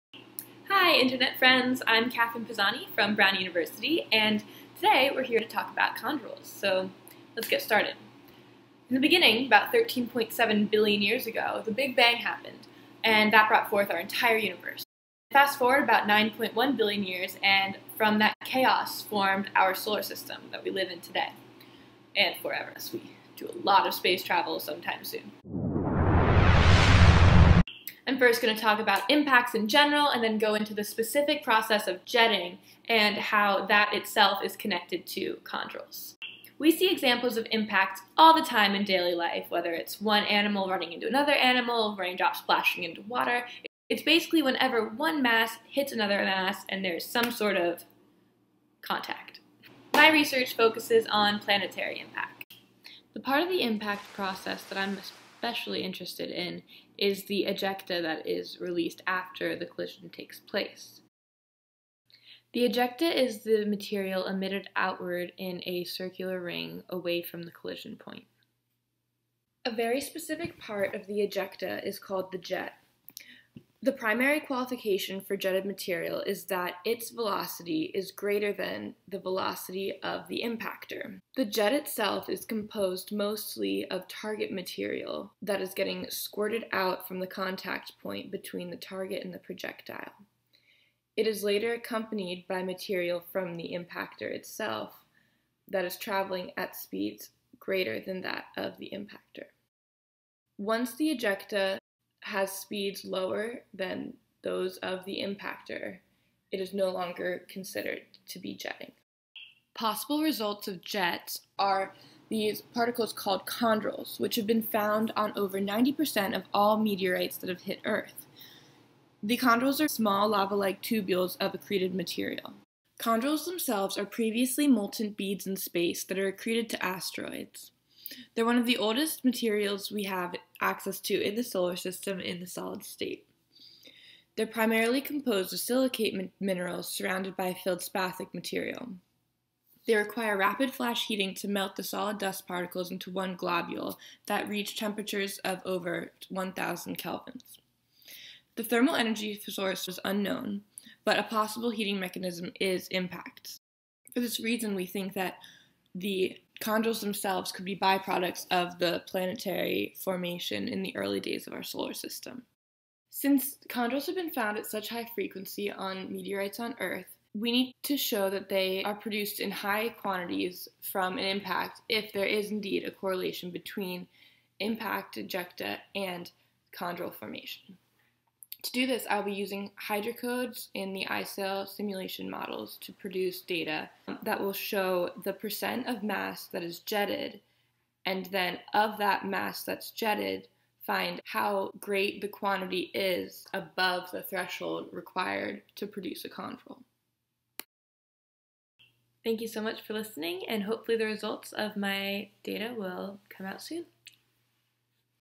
Planetary Impact, Impact Jetting and Chondrules in less than 5 minutes
This video is a brief and simple explanation of planetary impacts, impact jetting and the origin of chondrules. Its purpose is to show the significance of chondrules and their role in understanding the history of our solar system.

This video is for educational use only. The images, clips and graphics used may be found at the following links:
17. - Chelyabinsk meteor in Russia
18. Ehman.org- chondrule image
19. Meteorite-times.com
24. iSALE-2D shock physics code (Wünnemann et al., 2006), based on the SALE hydrocode solution algorithm (Amsden et al, 1980). To simulate hypervelocity impact processes in solid materials SALE was modified to include an elasto-plastic constitutive model, fragmentation models, various equations of state (EoS), and multiple materials (Melosh et al., 1992; Ivanov et al., 1997). More recent improvements include a modified strength model (Collins et al., 2004), a porosity compaction model (Wünnemann et al., 2006; Collins et al., 2011) and a dilatancy model (Collins, 2014).